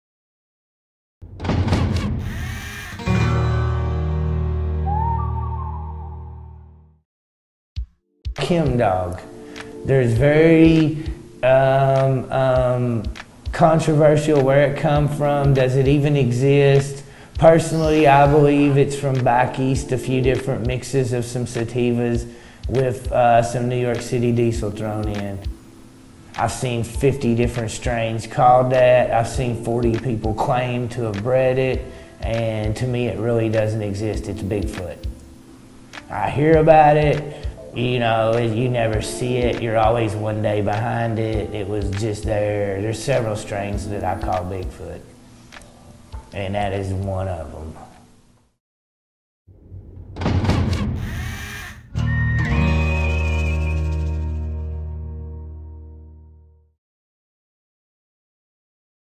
Chemdawg Marijuana Cannabis Seeds
Chemdawg Cannabis Seeds is what Steve Tuck (Horticulturalist / Botanist) likes to call a "Bigfoot" strains. These cannabis strains are elusive and their origins are still up for debate.